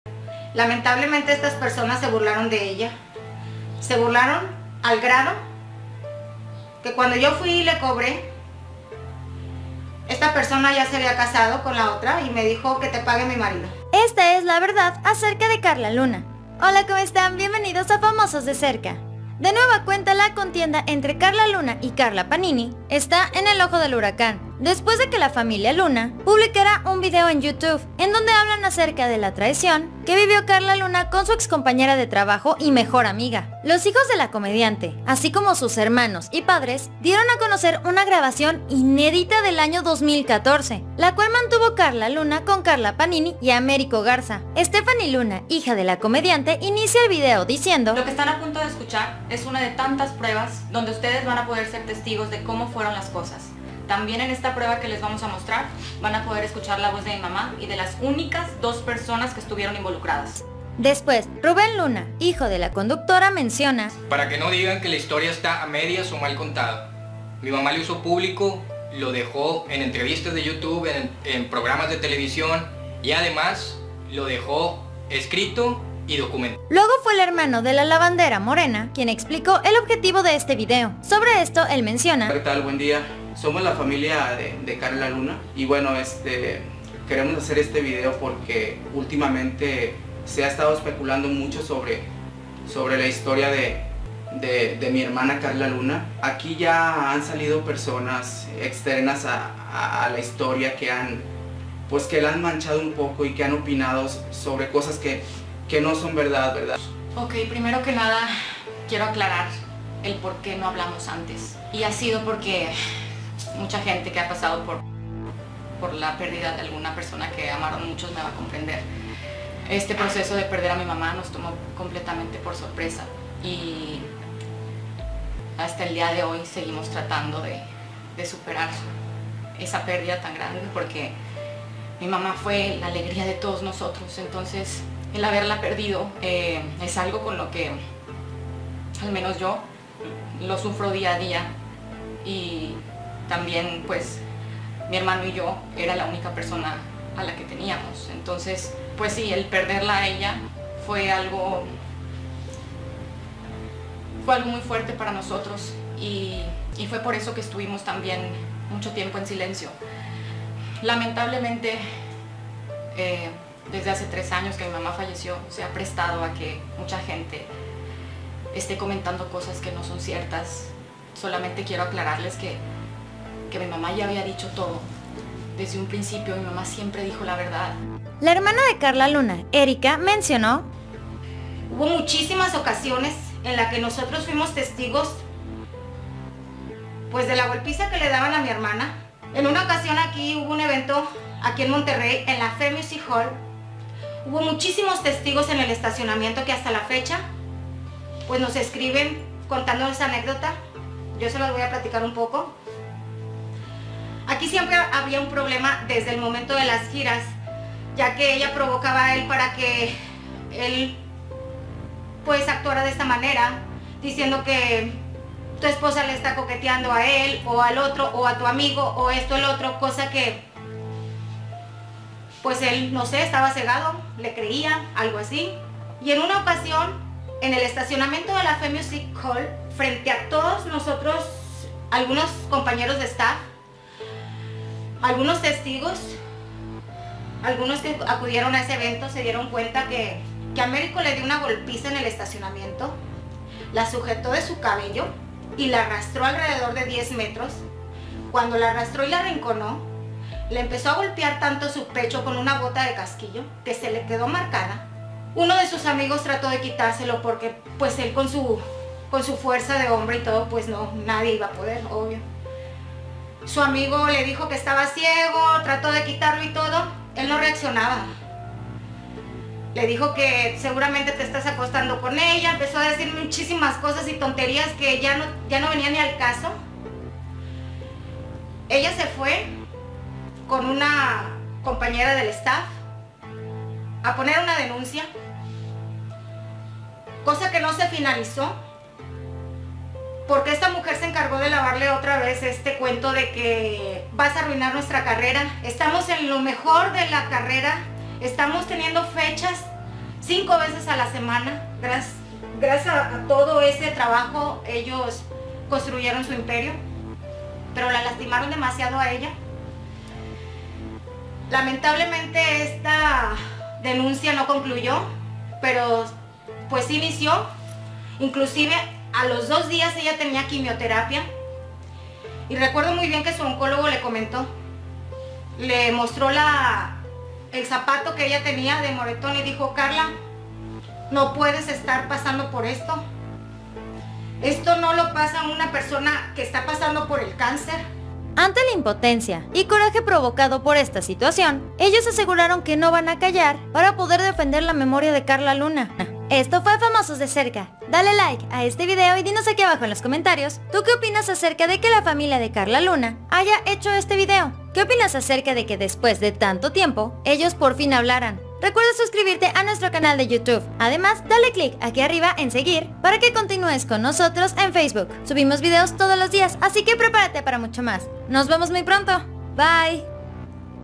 ¡ En EXCLUSIVA! ¡ Familia de Karla Luna Arremeten contra Karla Panini Revelan toda la Verdad !
La familia de Karla Luna no pudo mas y revela la verdad de la situacion de Karla Panini y Americo Garza no creerás todo lo que dijeron...

 BIENVENIDOS!!!